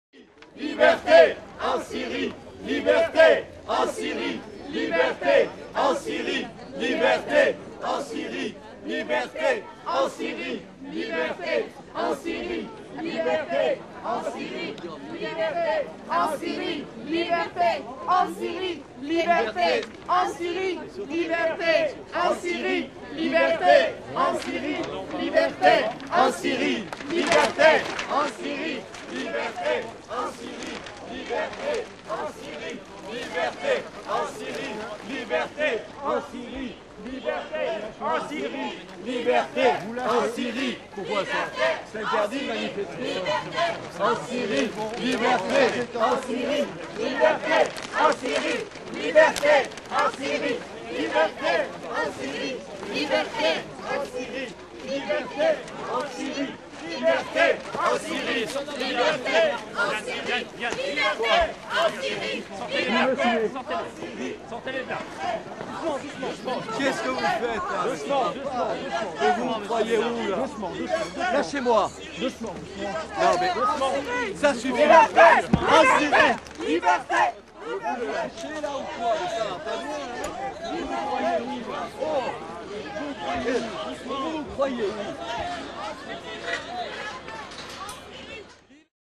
Head of Reporters Without Borders gets arrested for protesting in Paris
The head of Reporters Without Borders (RSF), Robert Menard, was arrested for protesting the presence of Syrian President Bashar al-Assad at the Bastille Day military parade in Paris.